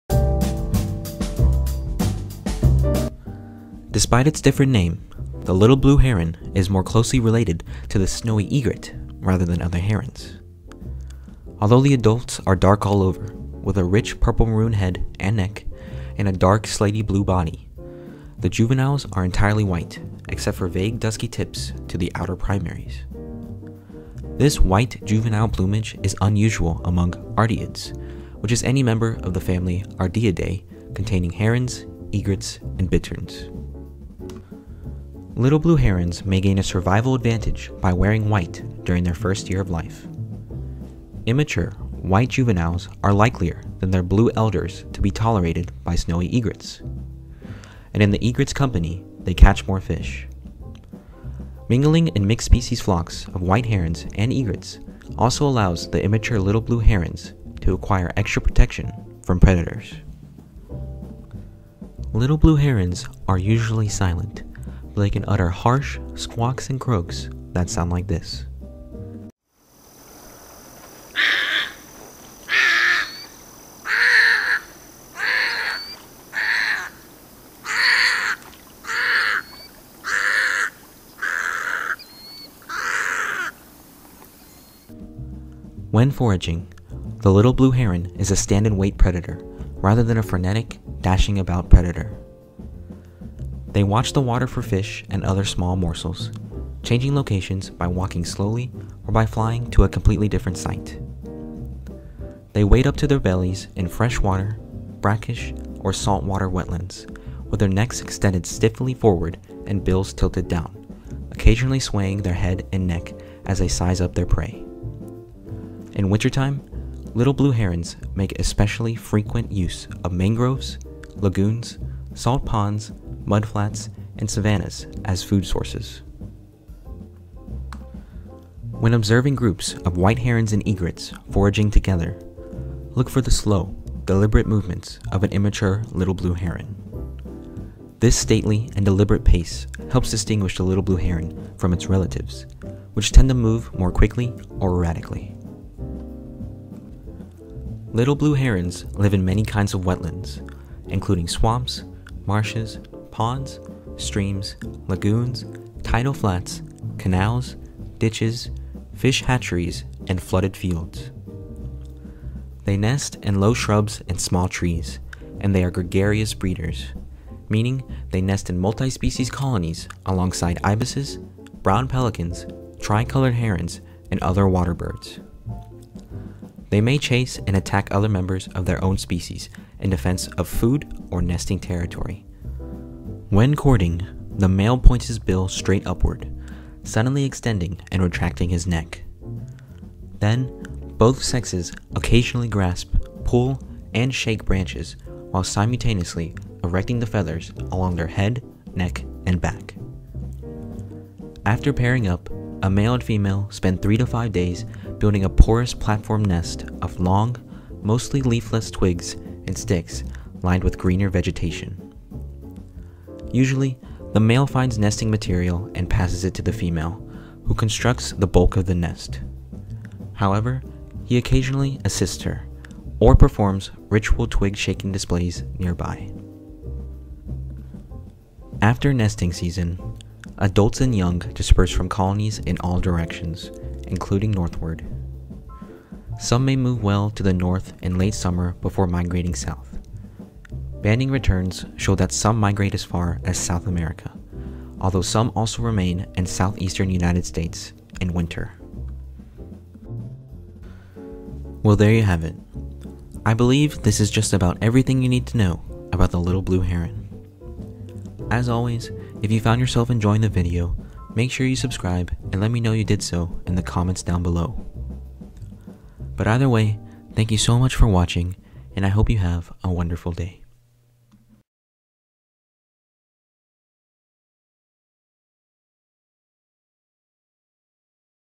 The Little Blue Heron: Everything You Need To Know | Courtship, Range, Hunting, Call/Sound, Flying
Next up on the Everything You Need To Know series is the Little Blue Heron! I hope y'all enjoy all the general facts I put in this video and I hope y'all learn something new. As always, let me know how I'm doing in the comments down below and if I'm forgetting anything. It's free and it'll really make my day :)

Thumbnail Photo: Geoff Coe